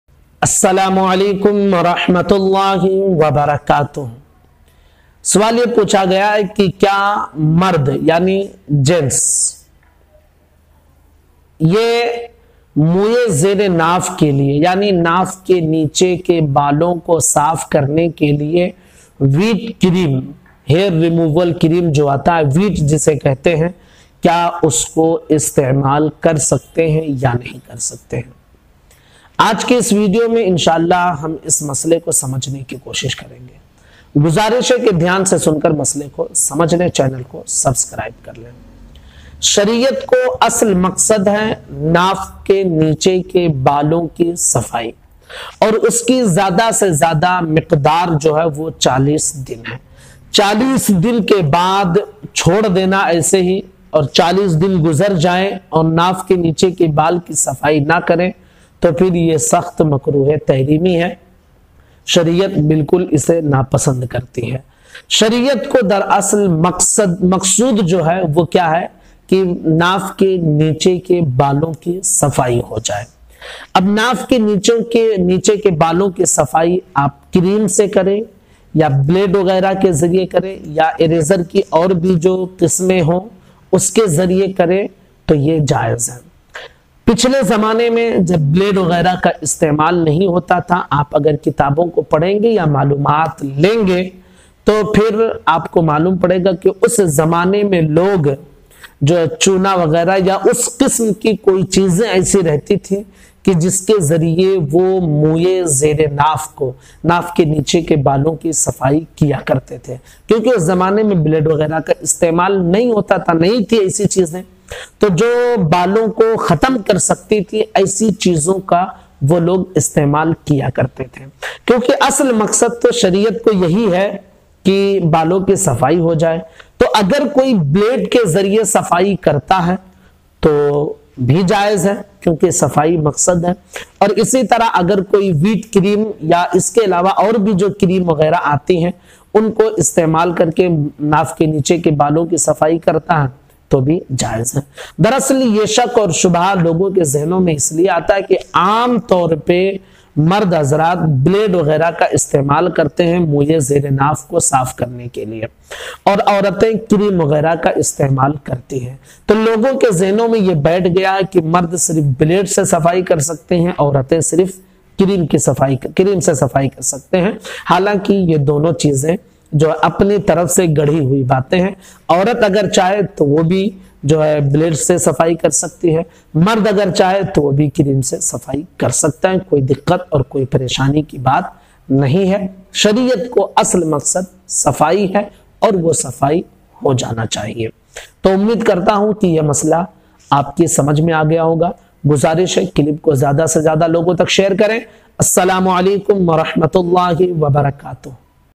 Kya Mard (Male) Naaf Ke Neeche Ke Baal Cream Se Saaf Kar Sakte Hain? Hair Removal Cream Use Karna?
Aaj ham aap ko batayen ge ki
Kya mard (Male) Naaf Ke Neeche Ke Baal Cream Se Saaf Kar Sakte Hain
Aap se guzarish hai ki is video ko Zaroor sune aur apni maloomat me izafa Karen aur apne doston ko bhi Hamare channel ke sath joren taki un ko bhi Deeni knowledge hasil ho